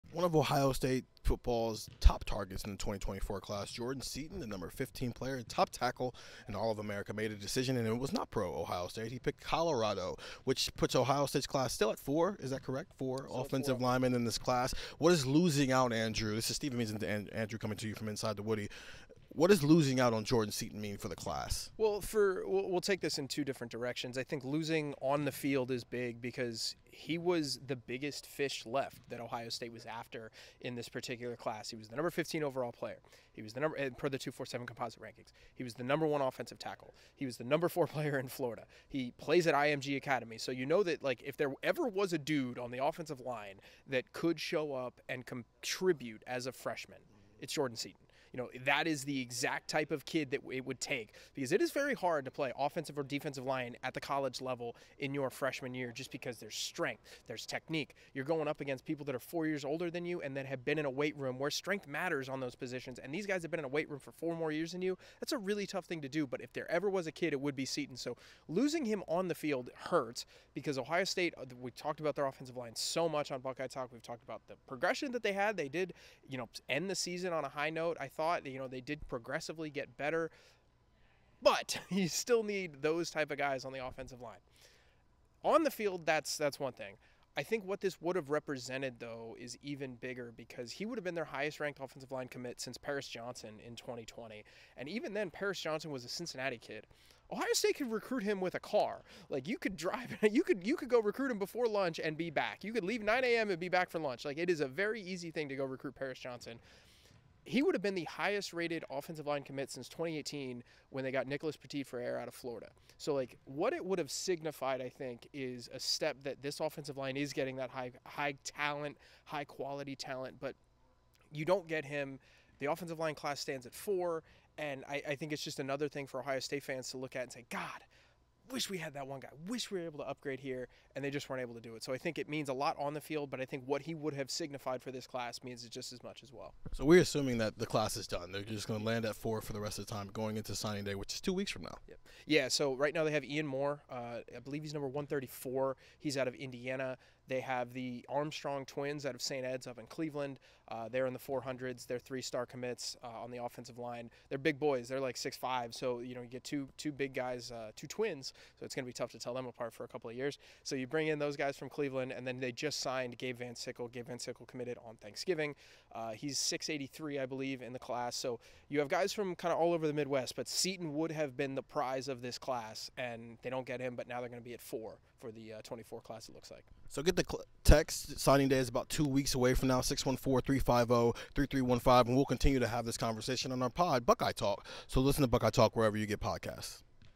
What 5-star Jordan Seaton committing to Colorado means for Ohio State football
Stephen Means and Andrew Gillis discuss Ohio State losing 5-star offensive lineman Jordan Seaton who committed to Colorado.